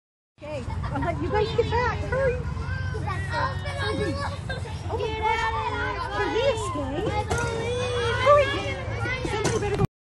Elephant at topeka zoo tried to escape!!!
Free the poor elephants!